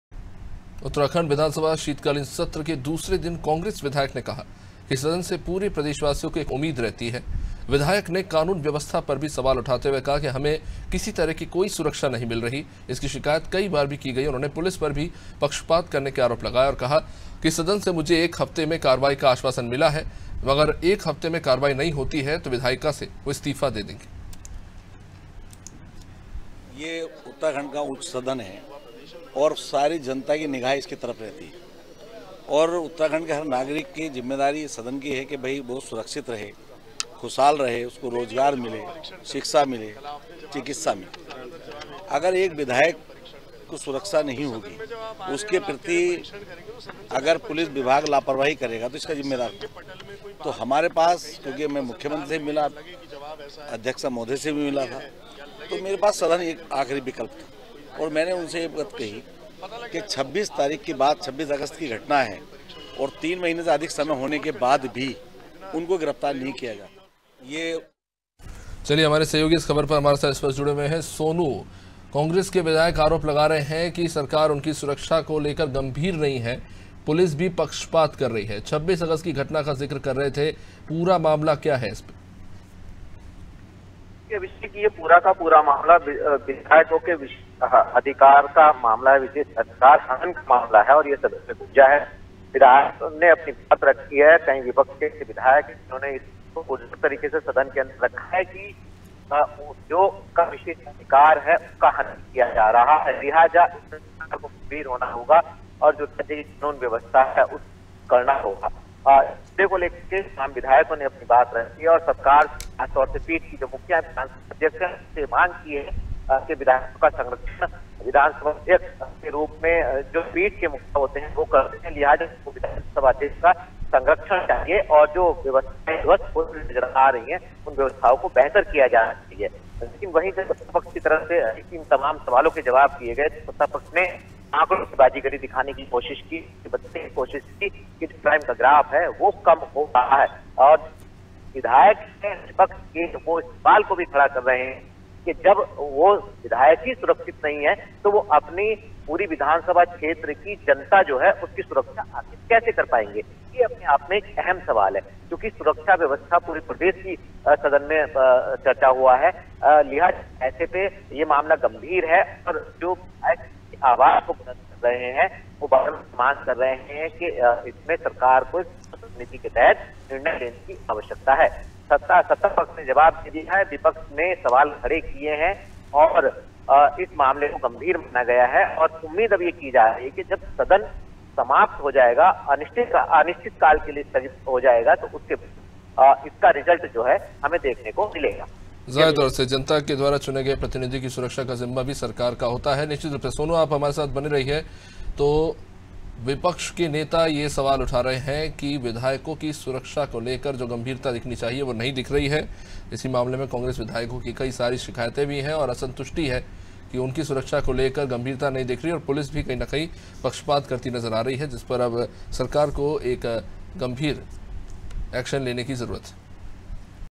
Dehradun : शीतकालीन सत्र का दूसरा दिन, सदन से प्रदेशवासियों को उम्मीद
उत्तराखंड विधानसभा शीतकालीन सत्र के दूसरे दिन कांग्रेस विधायक ने कहा कि, सदन से पूरे प्रदेशवासियों को एक उम्मीद रहती है...विधायक ने कानून व्यवस्था पर सवाल उठाते हुए कहा कि, हमें किसी तरह की कोई सुरक्षा नहीं मिल रही...जिसकी शिकायत हमने कई बार की है...उन्होंने पुलिस पर पक्षपात का आरोप लगाया है..आगे कहा कि, सदन से मुझे एक हफ्ते में कार्रवाई का आश्वासन मिला है...अगर एक हफ्ते में कार्रवाई नहीं हुई तो, विधायिका से इस्तीफा दे दूंगा...